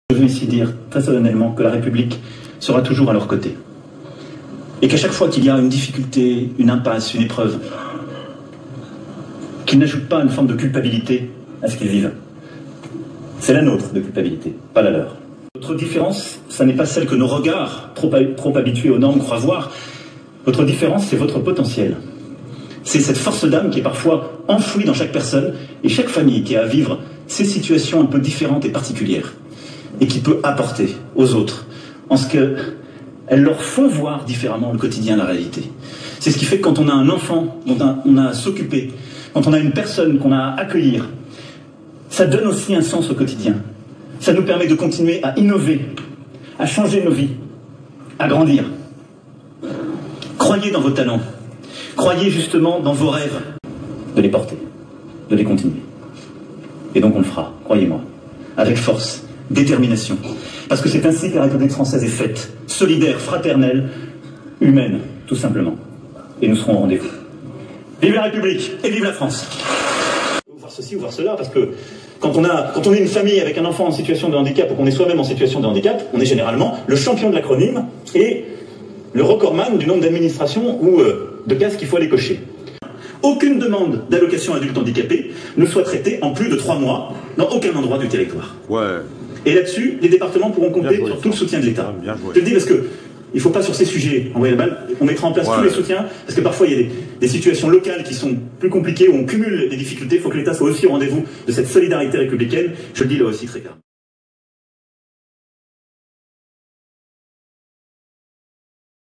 Monsieur le Président Emmanuel Macron et l'handicap {encore mal vu, pourtant...Belles paroles ?}
Si des curateurs se permettent des acronymes si l'handicap est encore jugé ici et là.
La MDPH mets plus de trois Mois à arriver.

Que reste t'il de vos paroles Monsieur le Président ?
Une question de département où bien de Region ?
Nos enfants placés nos enfants n'ont rien à manger nos enfants sont jugés que reste t'il de vos belles paroles.

Nos enfants n'ont pas de quoi commencer leurs vies !
Que reste t'il de vos belles paroles ?

il y aura t'il une réponse seulement ?